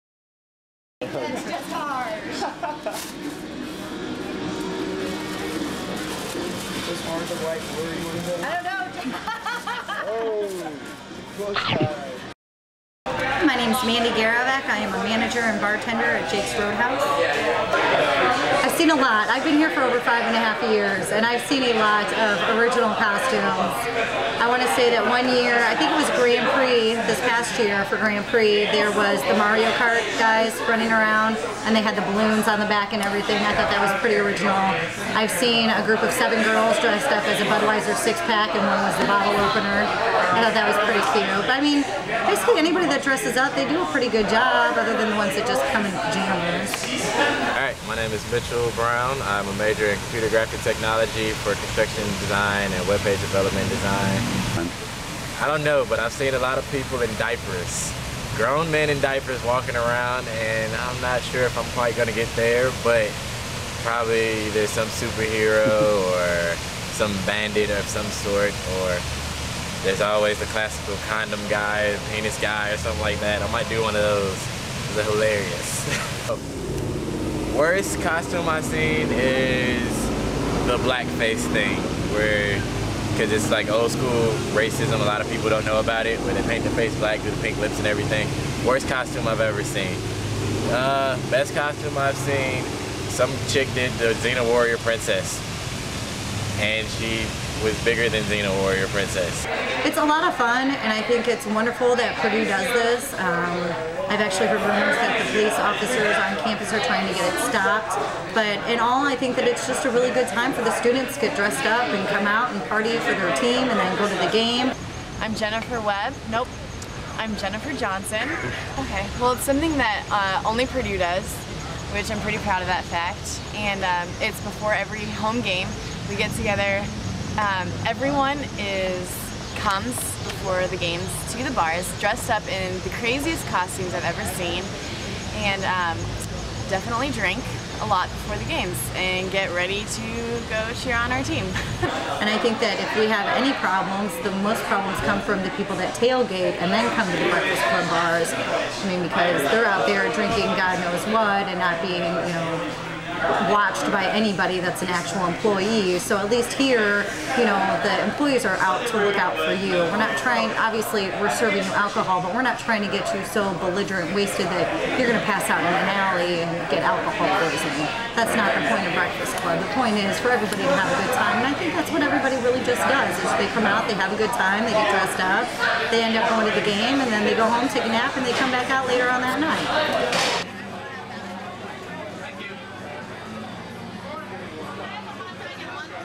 Breakfast Club
Students and bar owner discuss the Purdue tailgating phenomenon know as Breakfast Club.